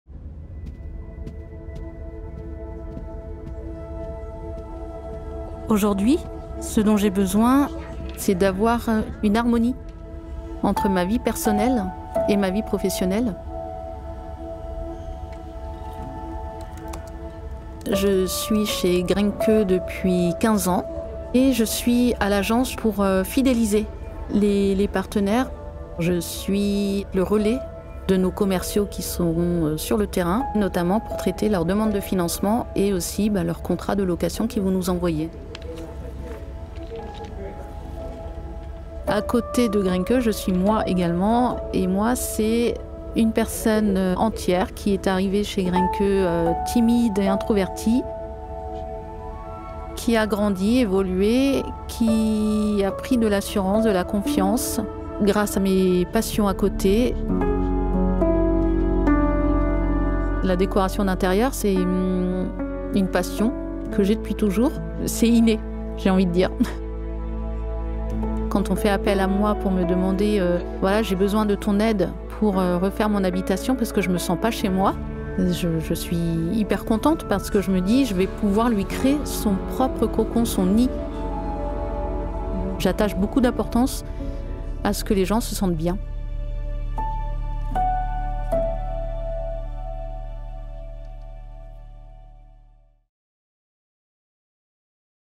Grenke | Portraits de collaborateur - Aurélie
Conception et production de 3 portraits pour la marque employeur de Grenke France.
Rencontre avec Aurélie, asssistante commerciale de l'agence de Strasbourg.

Production : Quentin Cogitore - Studio Centurion
Réalisation : Florian Coulon et Quentin Cogitore
Image : Bruno Fagotti
Son : Vivien Roche